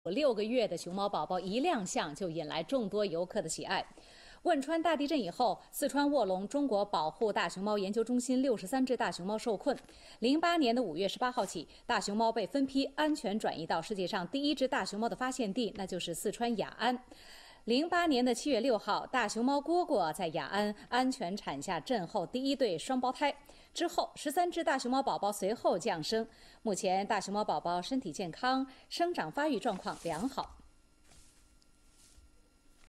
Raw Video: Panda Cubs Send New Year's Greetings
Thirteen giant panda cubs born in the heart of China's earthquake zone sent their greetings for the Lunar New Year. The pandas were born in a province that saw a devastating earthquake in 2008 that left nearly 90,000 people dead or missing. (Jan. 28)